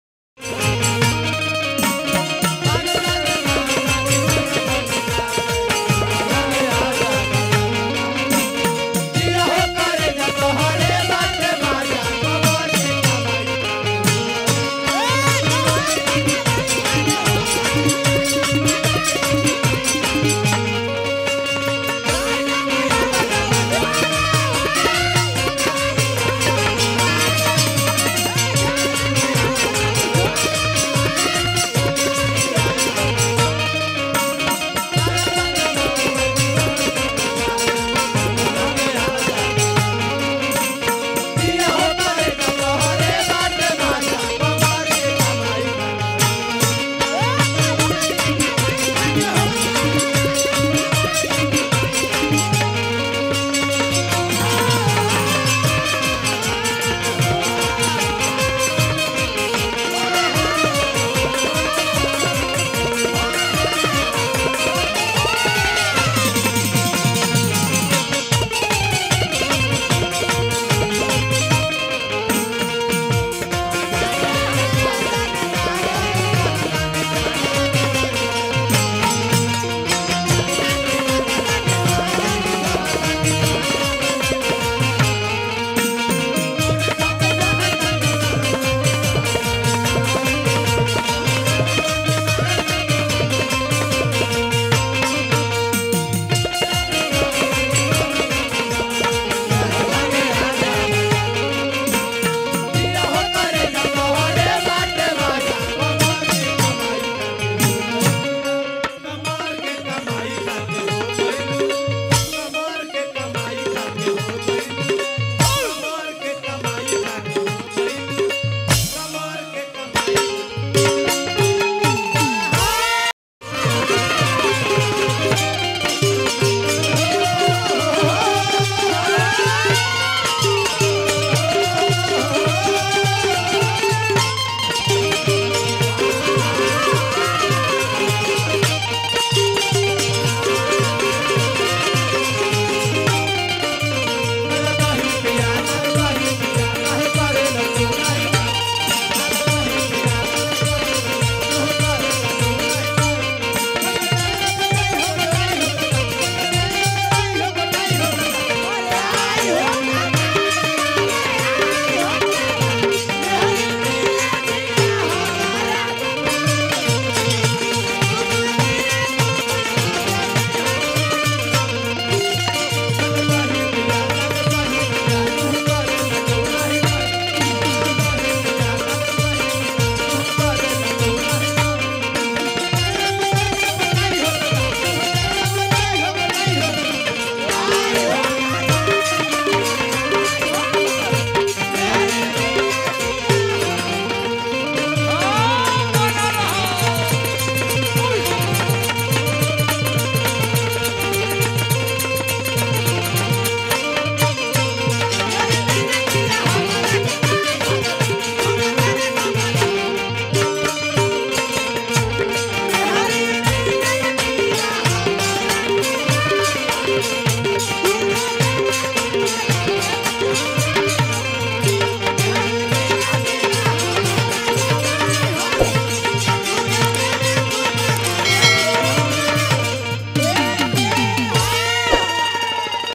गर्दा काबार लहरा डांसर के साथ गजब का डांस | Bhojpri Lahra | Dugola Chaita | Sudarshan Yadav
हेलो दोस्तों ,
इस चैनल पर नये - नये बिरहा , दुगोला और स्टेज प्रोग्राम देखे |